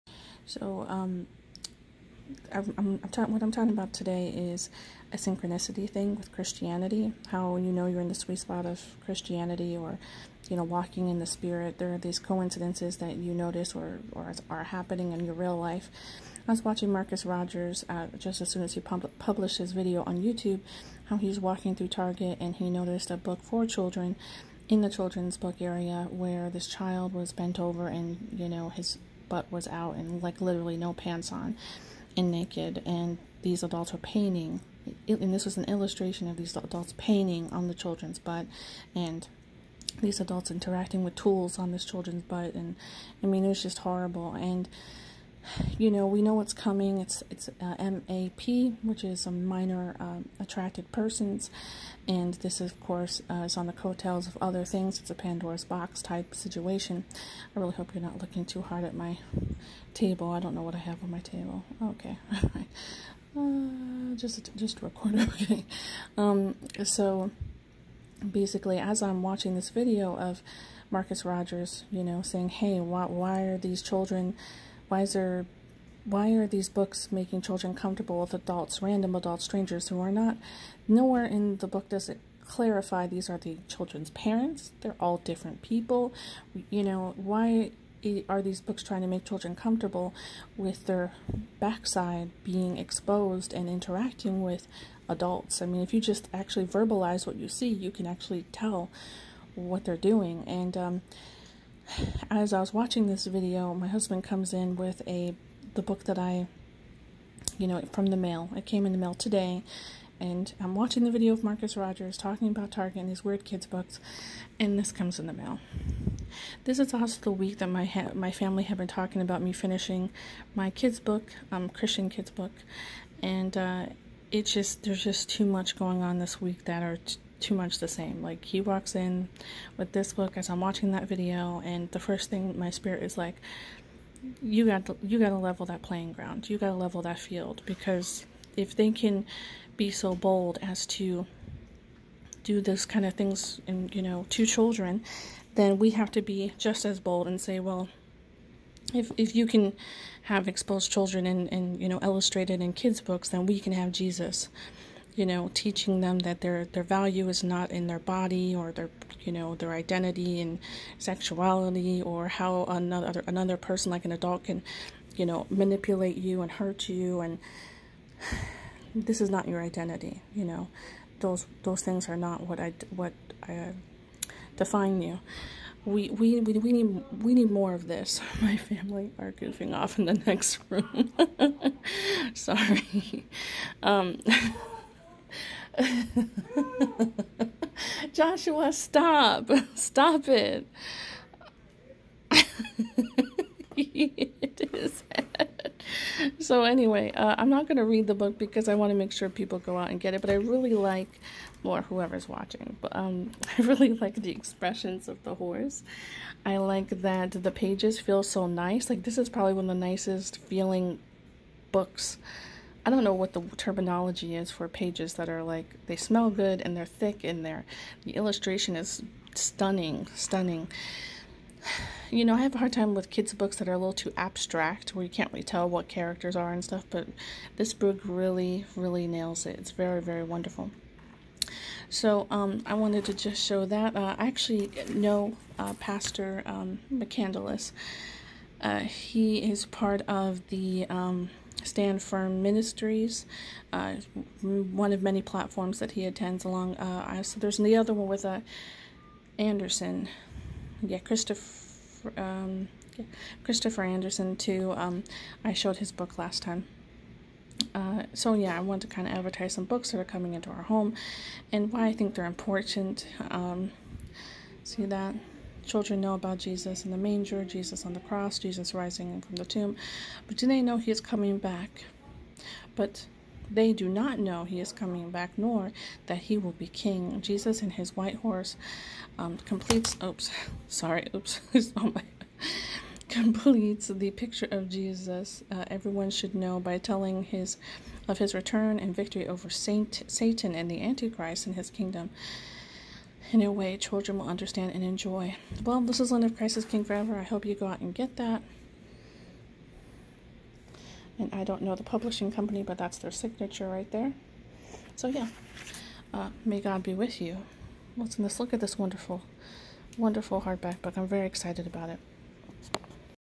Jesus and His White Horse
Jake Mccandless
Tyson Ranes

You can't find it on Amazon, this is where I found it:
I will update this link soon if I find a better one.

Did he change because his logically considered which religion would make him a good person?... or was he meant where he was with blood on his hands. He had no clue of difference between real light and artificial light until he was touched but the real thing.